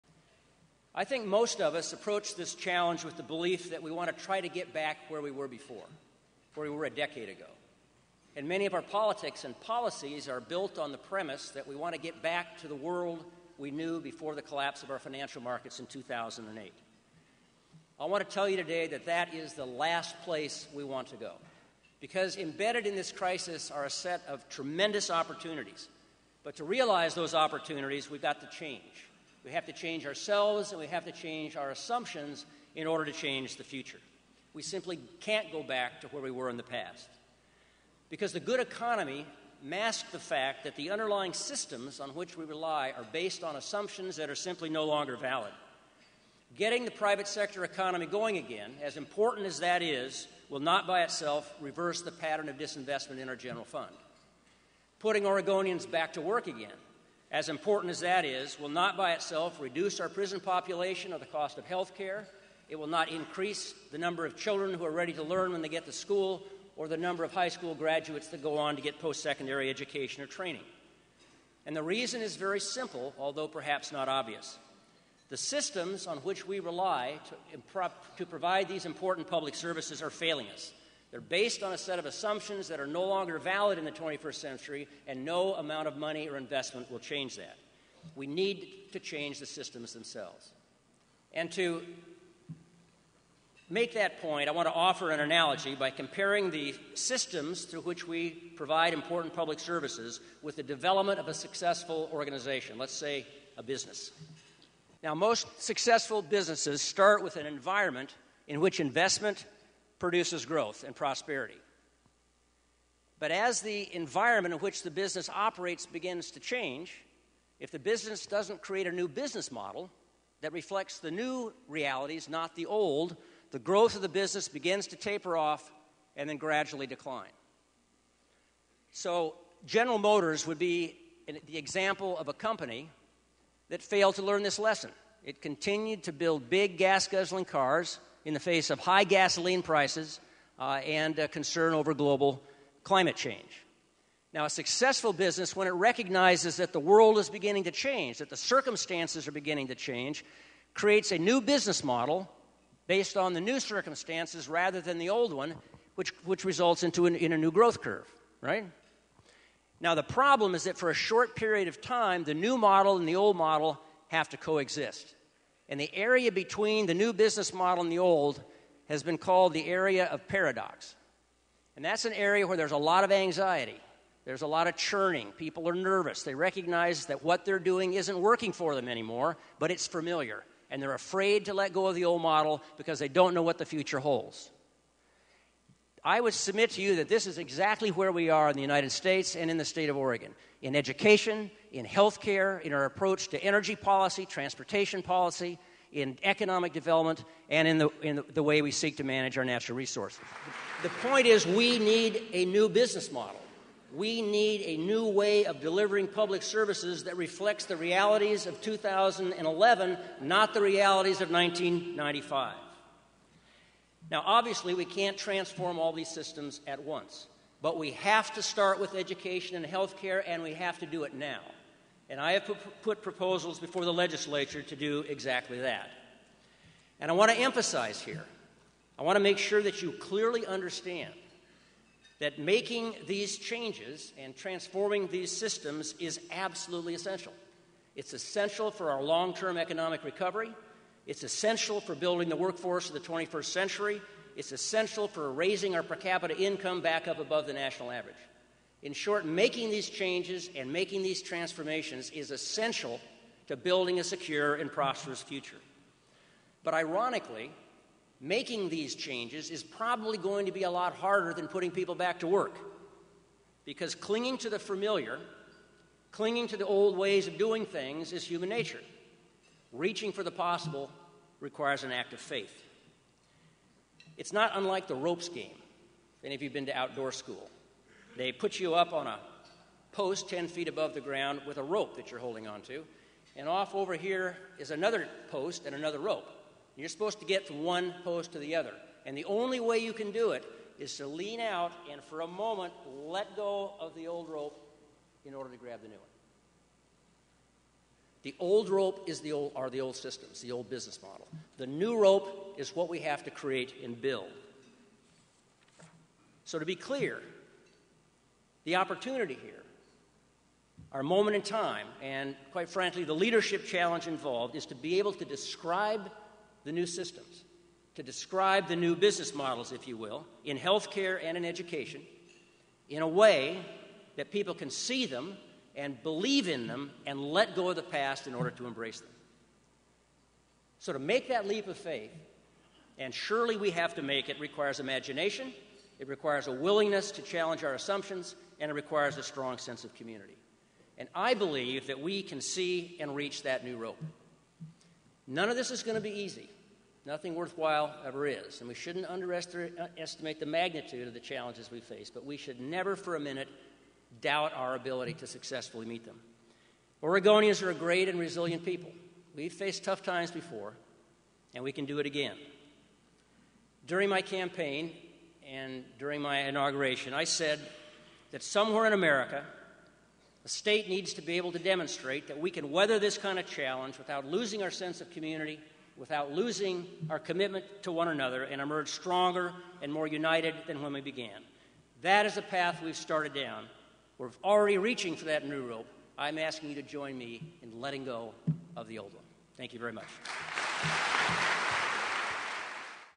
Governor Kitzhaber Speaks at City Club March 4
Governor Kitzhaber asks Oregonians to join him in "letting go of the old rope" and transform Oregon for the better.